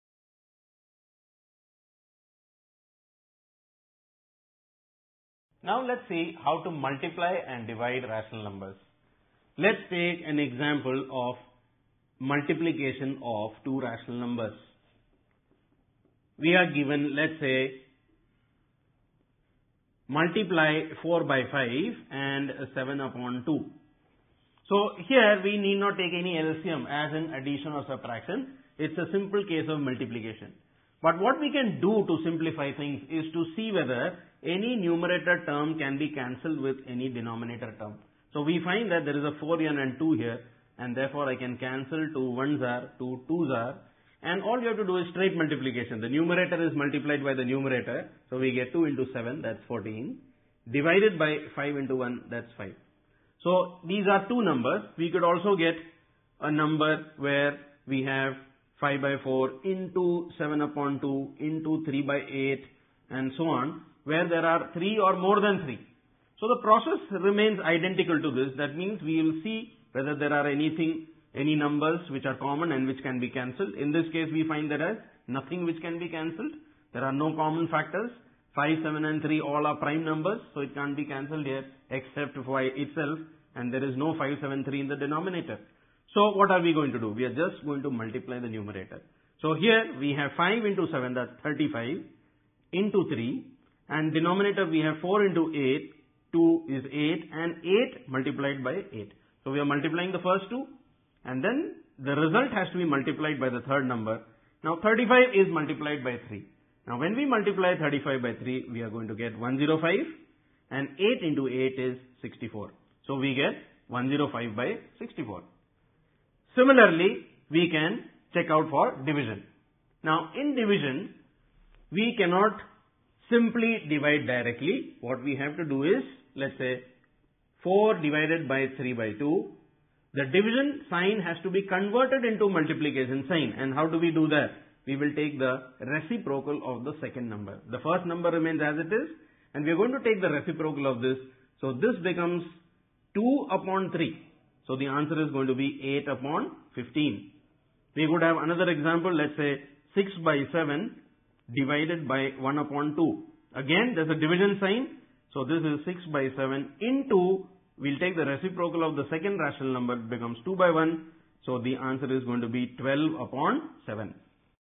CBSE class 8 - Chapter 1 - Part 6 - Multiplication & Division of Rational numbers
CBSE / ICSE / State board class 8th Math, Chapter 1 | Rational Numbers | Free Solutions | Study Notes | Question and Answers | Chapter Summary

This Video - Multiplication & Division of Rational numbers

For understanding this chapter watch the complete video series.